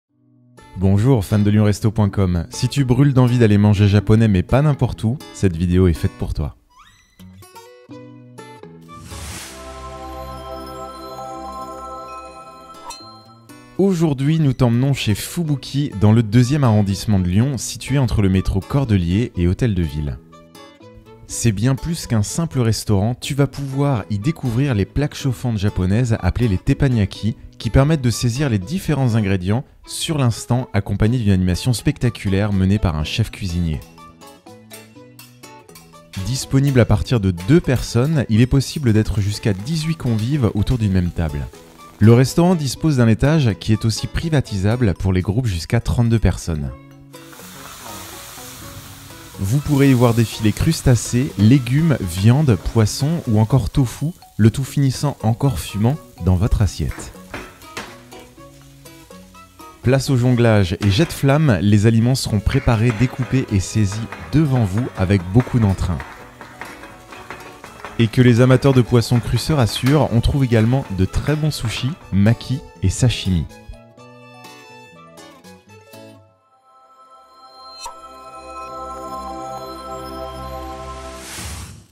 Restaurant Fubuki - Lyon - Spectacle culinaire
Maniant habilement les ustensiles, lui et ses cuisiniers offrent aux convives assis autour de la table, un spectacle impressionnant. Entre jonglage et jets de flammes, les aliments sont préparés devant les gourmets aux yeux grands ouverts. On peut voir ainsi défiler crustacés, légumes, viandes, poissons ou encore tofu, le tout finissant encore fumant dans notre assiette.